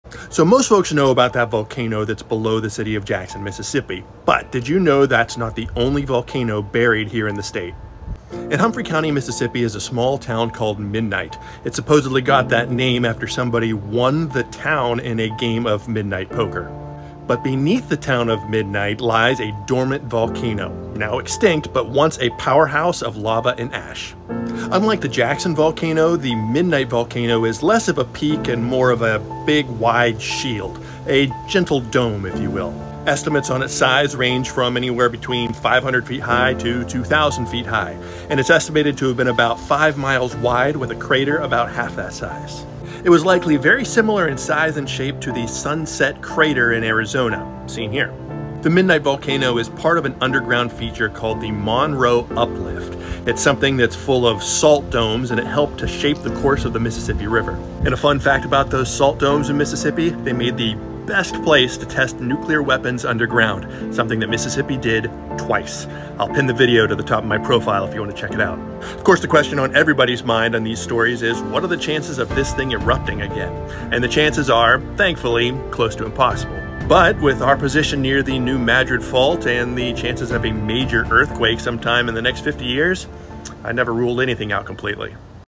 Mississippi’s Midnight Volcano
Mississippi has two extinct volcanoes. One is directly beneath the capital. The other lies beneath a small town called Midnight